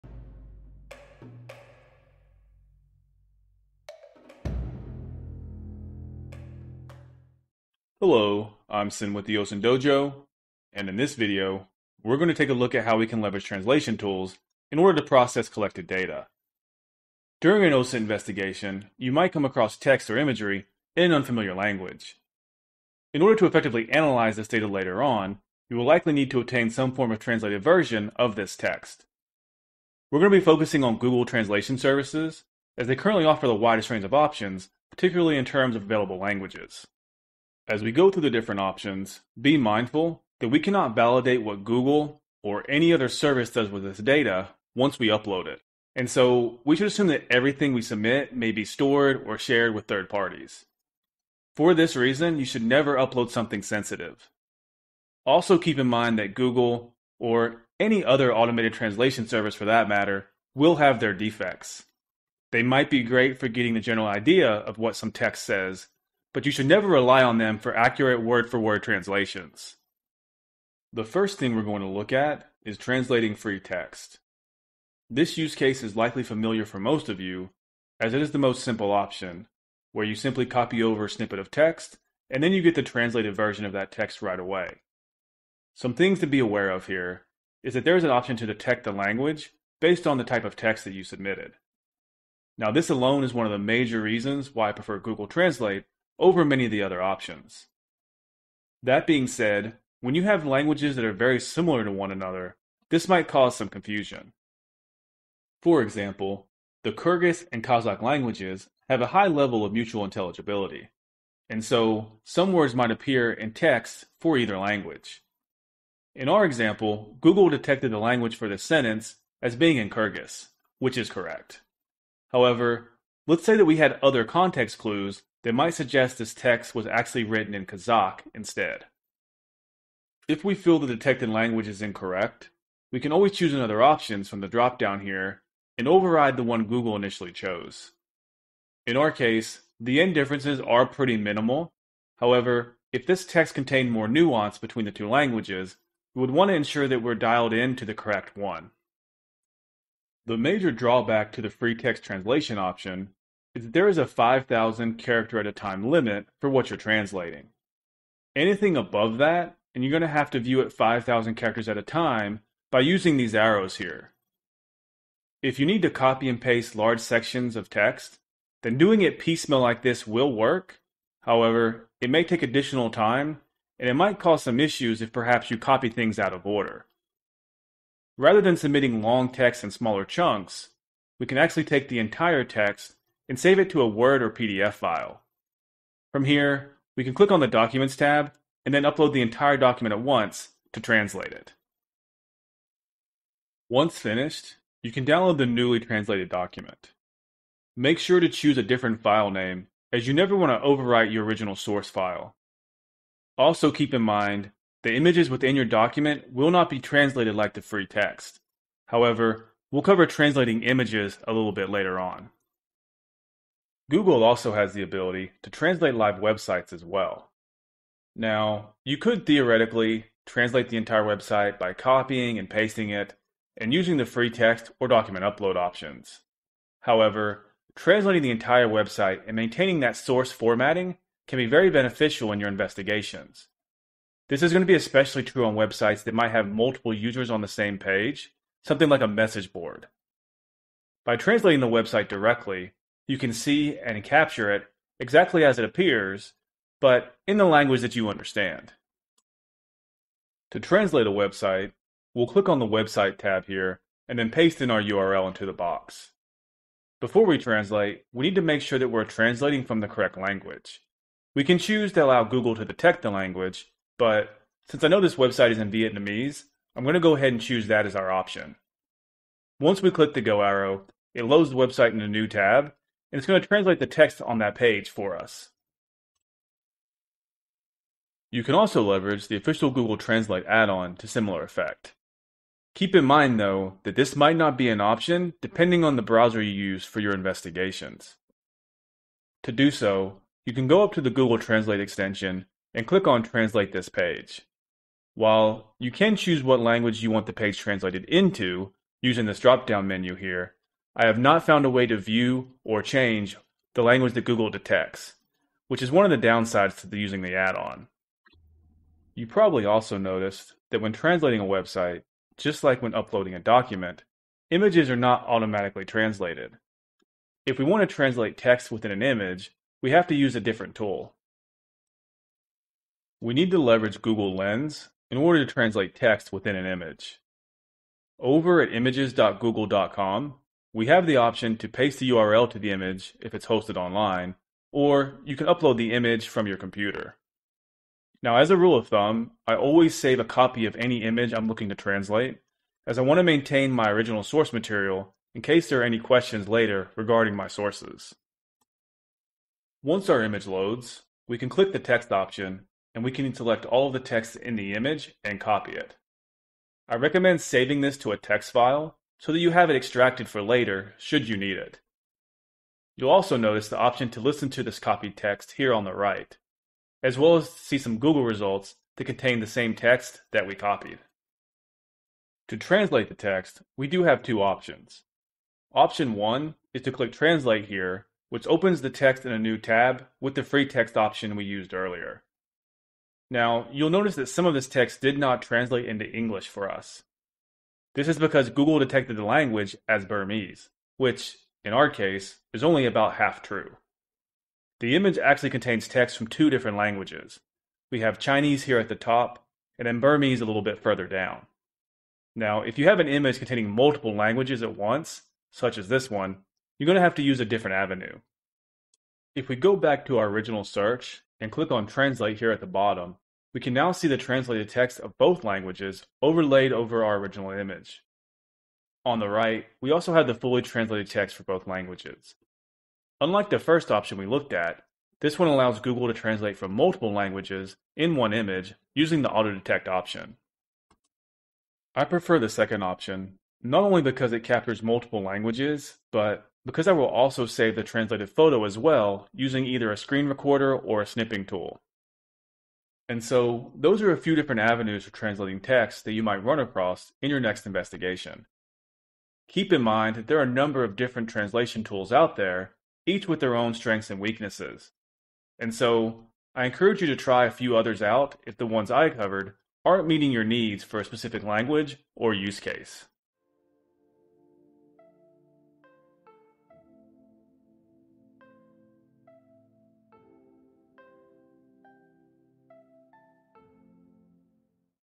OSINT Processing: Translating Text
In this video we take a quick look at the different Google Translate options for translating free text, documents, live websites, and images.

TIMESTAMPS
00:08 - Introduction
01:11 - Translating Free Text
02:35 - Translating Documents
03:33 - Translating Websites
05:24 - Translating Text Within Images